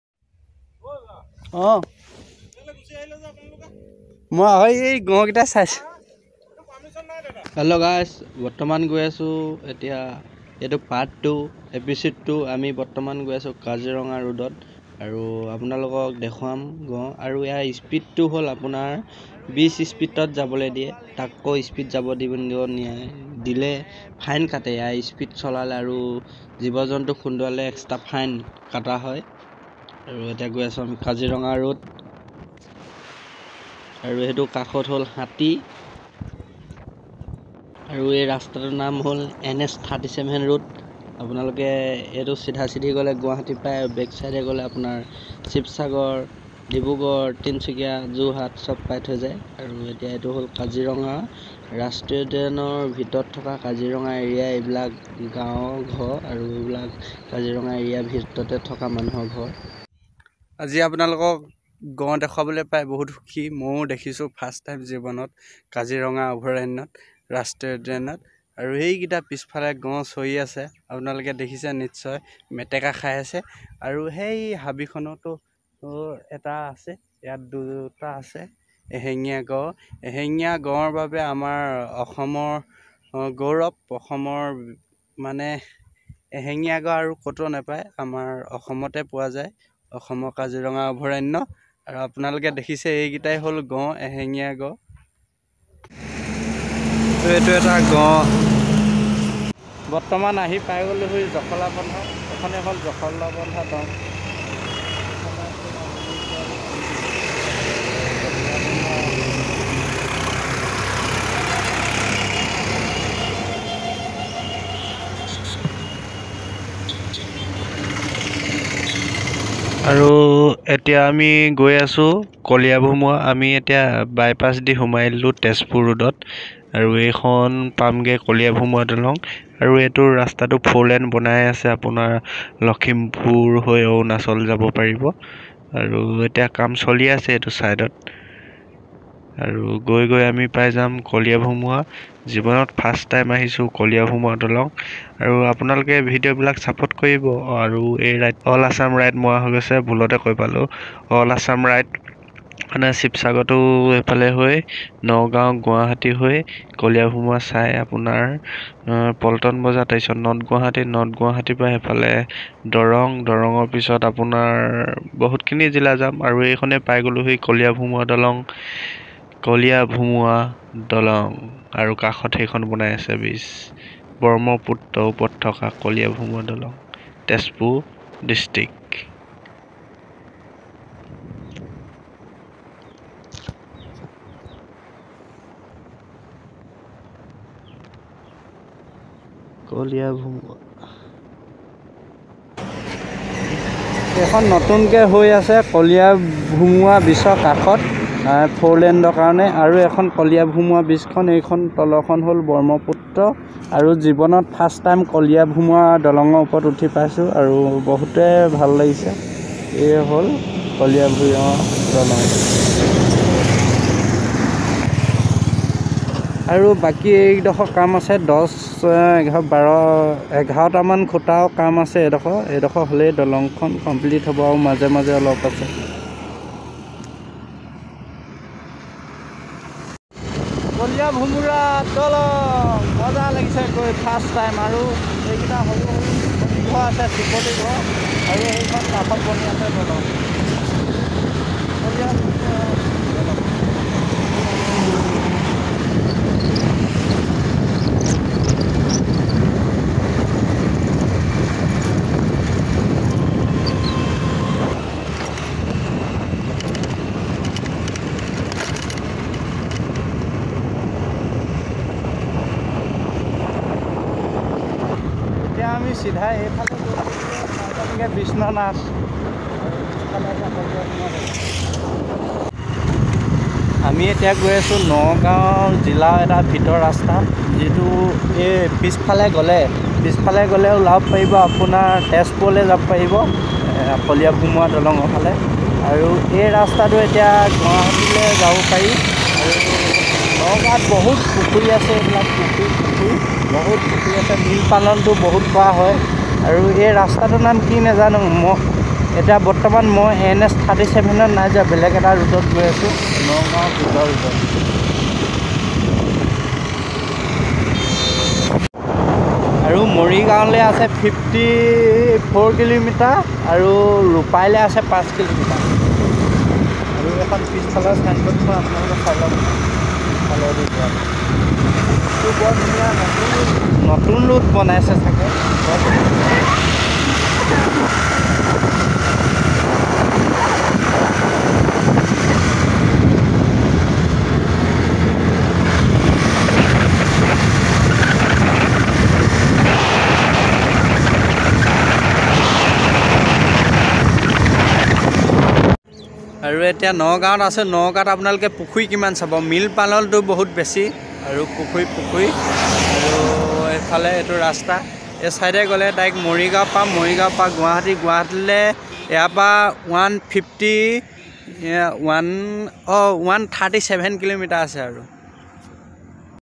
All Assam Scooty Ride | Kaziranga To Nagaon | Kaliabhomora Setu | Ep.02 | Honda Activa | Pranjal
Kaliabhomora Bridge

All Assam Ride
India. Sivasagar

Honda Activa 110cc